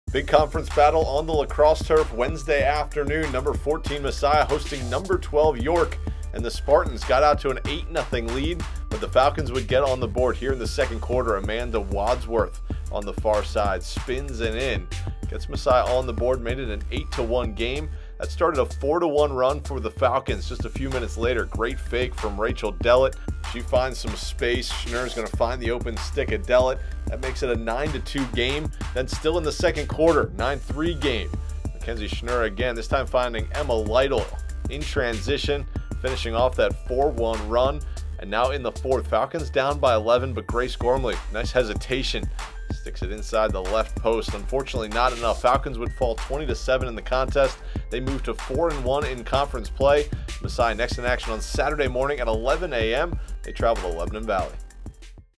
Messiah Women's Lacrosse Highlight of the Night - Apr. 20, 2022
The York Spartans earned a 20-7 victory over Messiah in a top-15 game at the Lacrosse Turf.